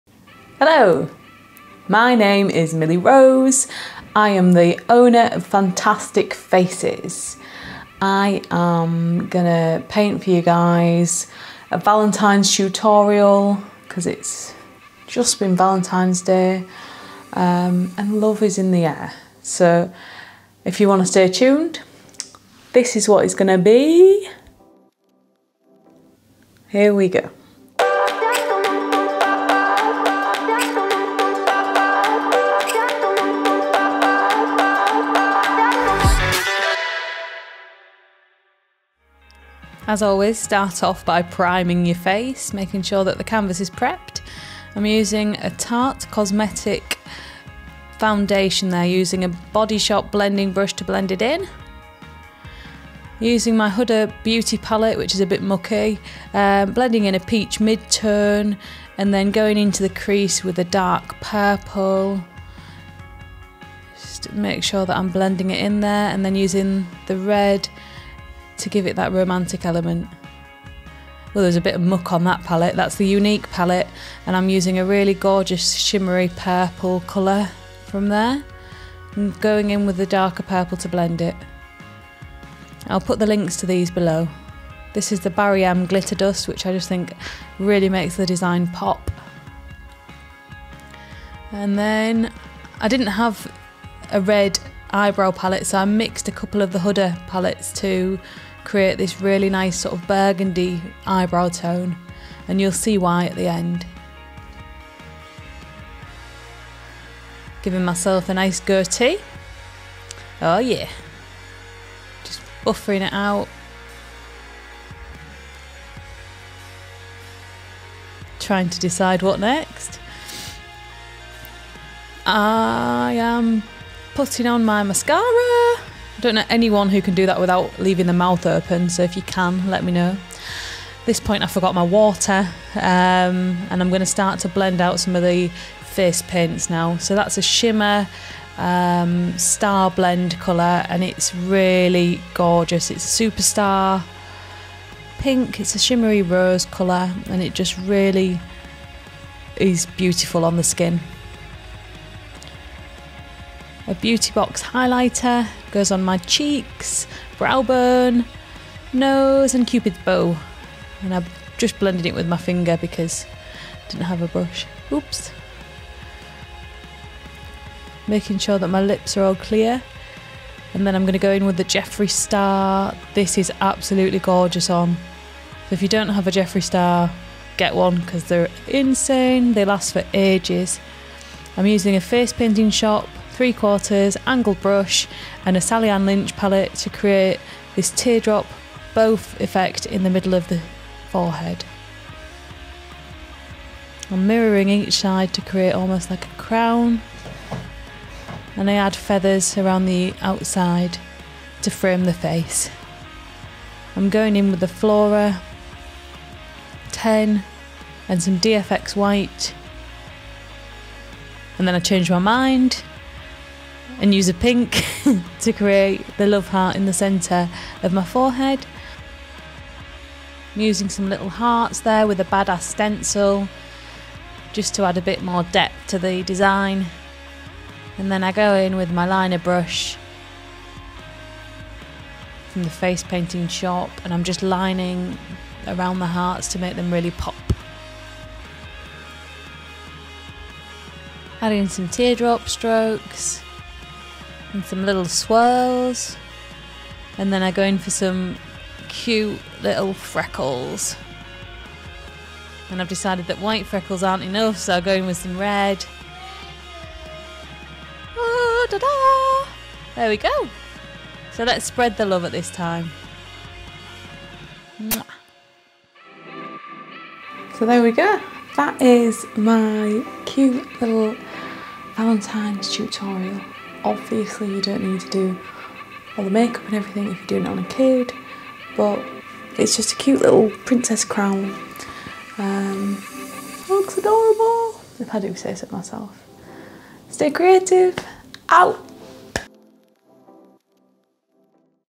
Face Painting a Love-Themed Design (Princess Crown)
Help spread the love with this Valentine's-themed, Princess Crown design - complete with voiceover, where I walk you through the stages needed to create this colourful piece. We will be posting more over the next few weeks so if you have any ideas drop them in the comments below creatingcolourandjoy facepainting # faceart